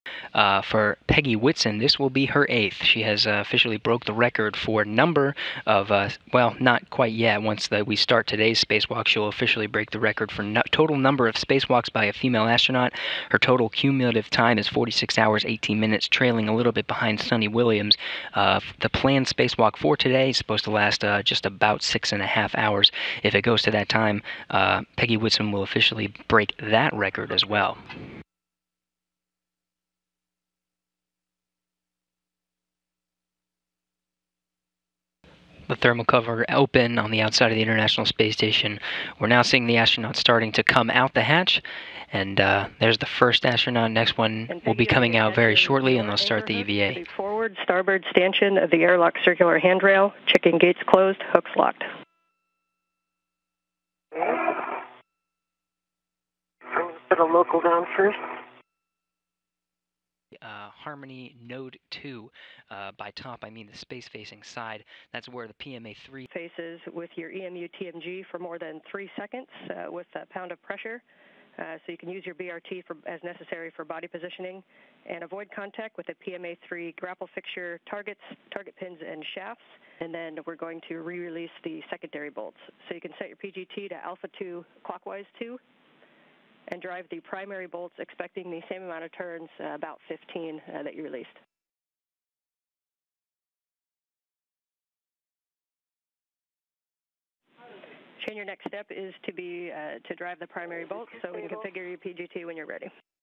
Oldest Spacewoman Sets Spacewalking Record
NASA astronaut Peggy Whitson floated out on the eighth spacewalk of her career Thursday morning, 250 miles up at the International Space Station. That's the most spacewalks ever performed by a woman. (March 30)